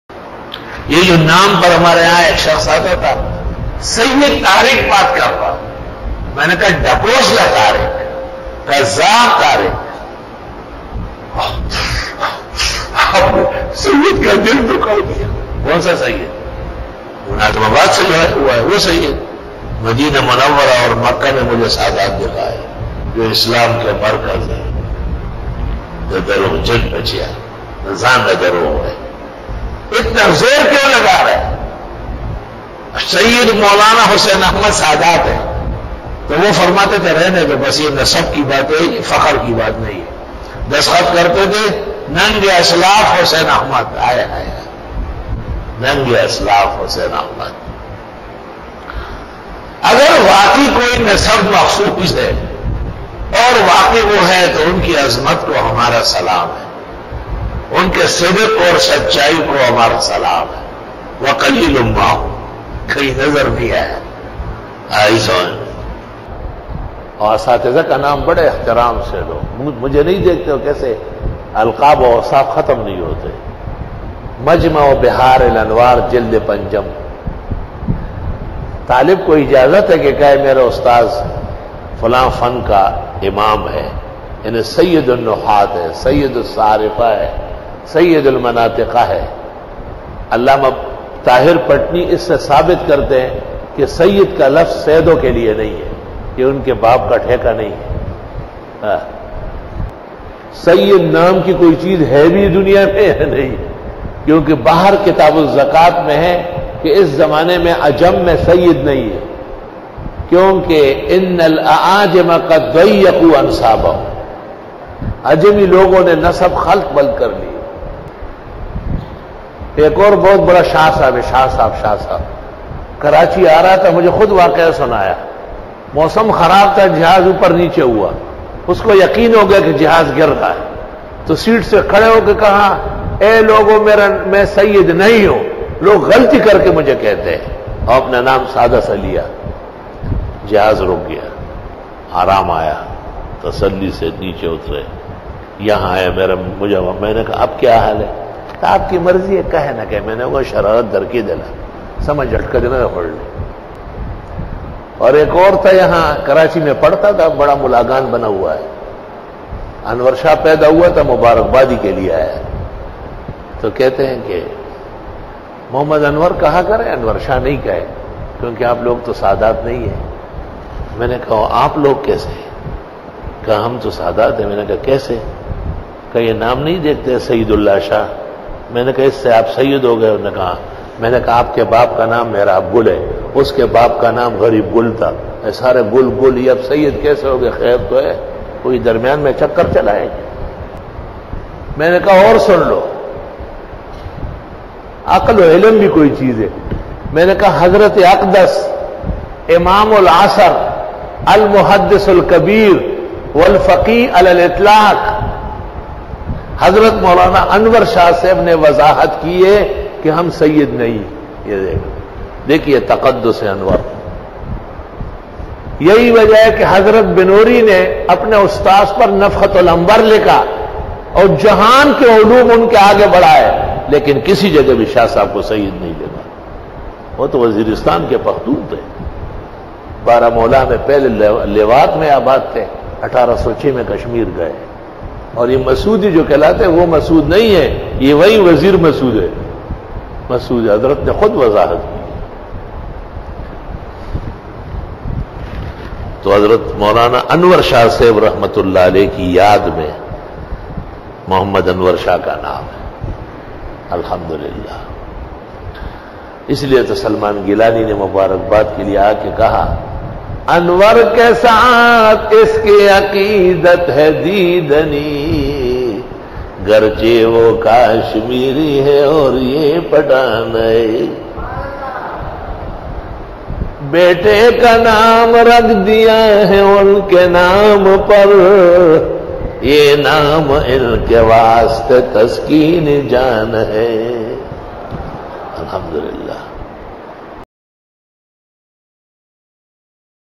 Realty of Sayed caste by Mufti Zarwali Khan
(Naam k sath sayed lagana)
kya sayed ka Lafz Sirf Sadat keliye ha?
Realty of Sayed caste
سیّد